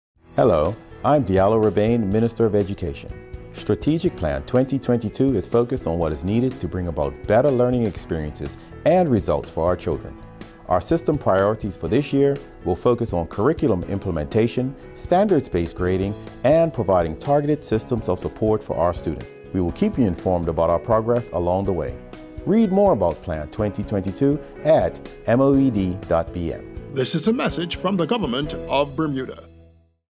The Hon. Diallo Rabain highlights Plan 2022
The Minister of Education, the Hon. Diallo Rabain highlights Plan 2022 - Bermuda’s Strategic Plan for Public School Education.